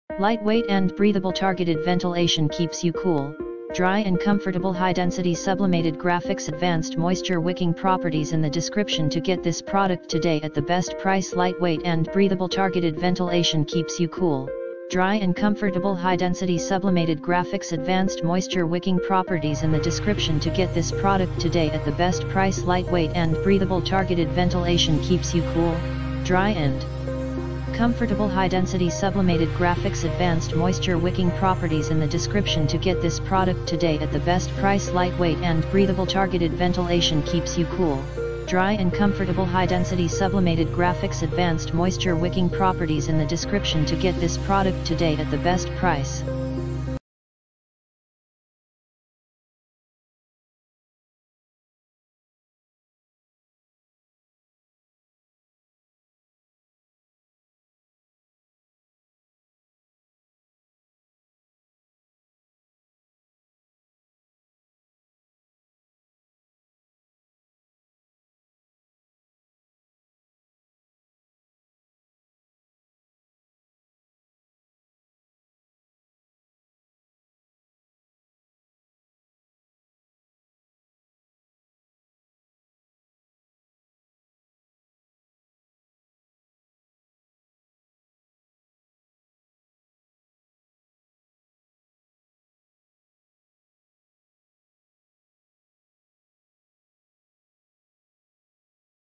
HK Army Freeline Paintball Jersey - Graphite
Light-weight & breathable
Targeted ventilation keeps you cool, dry and comfortable
High-density sublimated graphics
Advanced moisture wicking properties